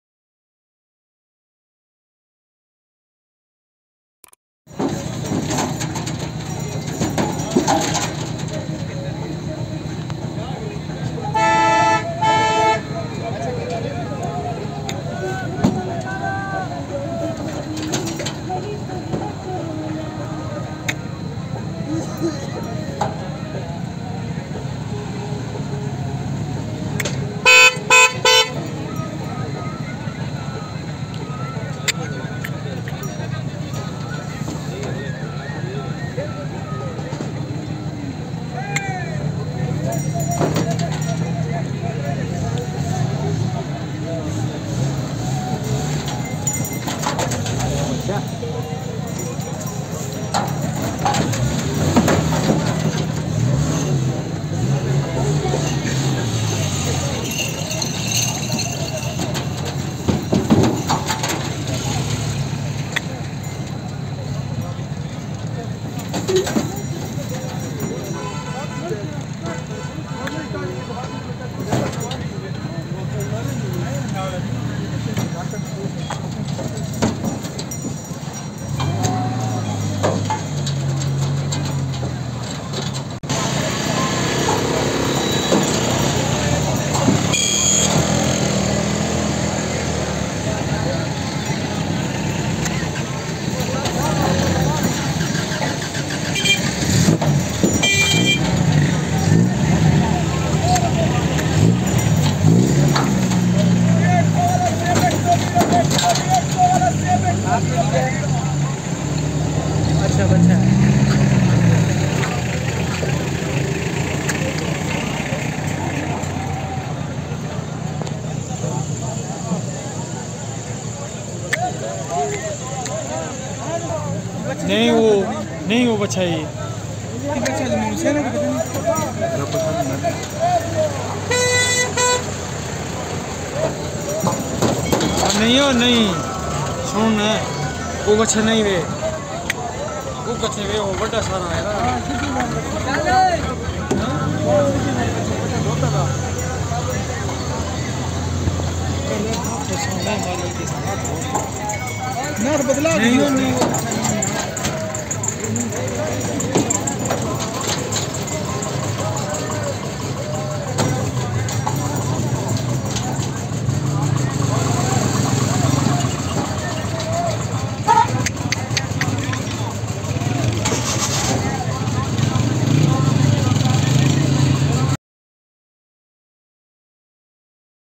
Uploading bull || domail mandi || Dangarous bull
Beautiful Dhani Breed bulls for sale on Domail mandi Fatahjang, punjab Pakistan.